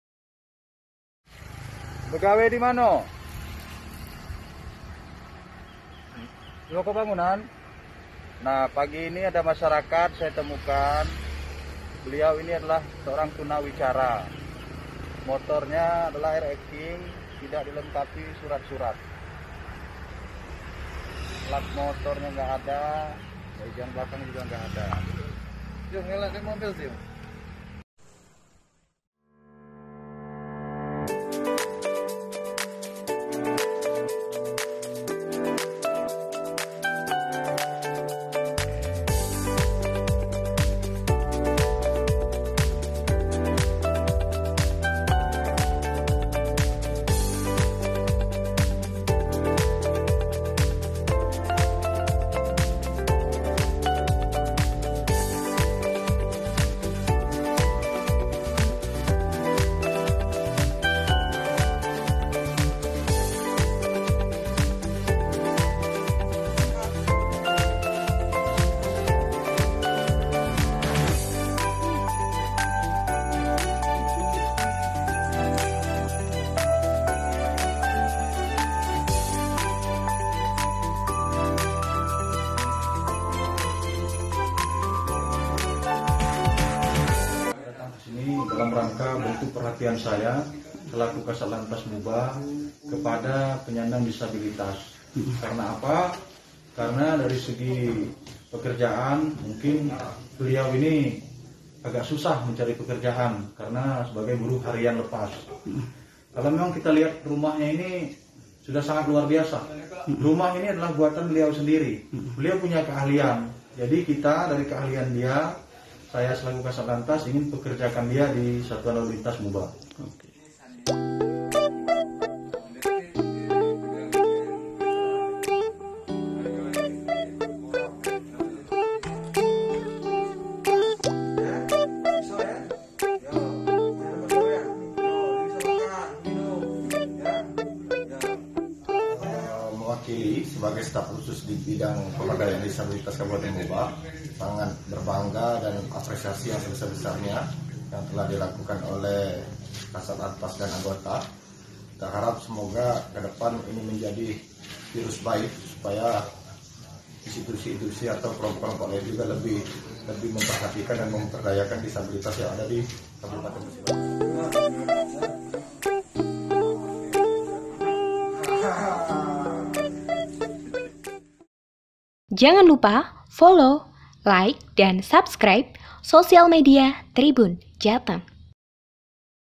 Viral Pria Difabel Dapat Pekerjaan dari Polisi Seusai Ditilang
Yasa (50) mendapatkan pekerjaan usai terjaring patroli rutin Polres Musi Banyuasin pada Jumat 16 Juli di kawasan Pasar Talang Jawa.

Yasa kedapatan tak memiliki surat kelengkapan, tak menggunakan helm, dan melawan arus.

Yasa merupakan seorang penyandang disabilitas, dia tidak memiliki kemampuan untuk berbicara.

Petugas kemudian mengajak Yasa ke Polres untuk diberi sosialisasi mengemudi dengan baik dan benar.

Mengetahui Yasa memiliki ketrampilan, Kasatlantas Polres Muba AKP Sandi Putra justru memperkerjakan Yasa di bagian Disatlantas.